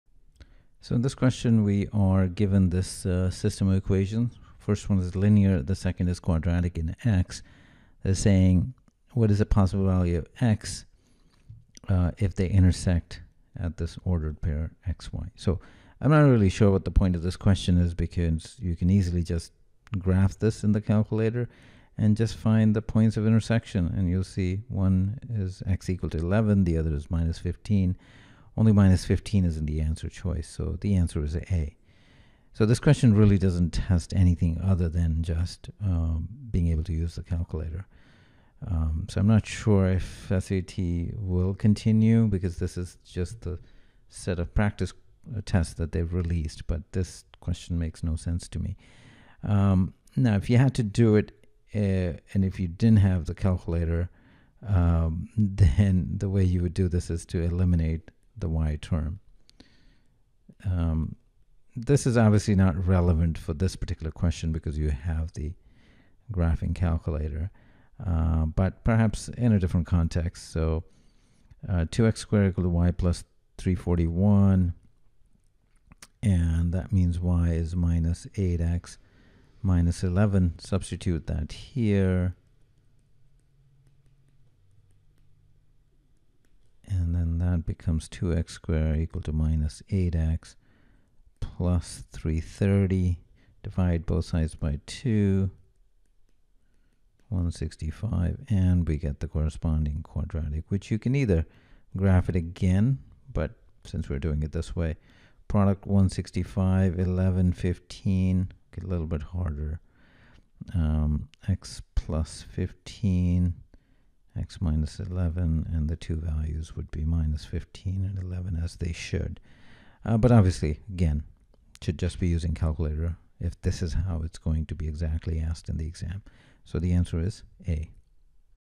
Question 11 Digital SAT Bluebook Practice Test 1 Section 2 Math Module 2 Difficult
Video explanations to all of the math questions in the official Digital SAT Practice Test 1 Math Module 2 Difficult that is part of the Collegeboard’s Bluebook app. You will receive this module if you performed above average in the first math module.

for video solutions to every single math question released in official SAT and PSAT tests.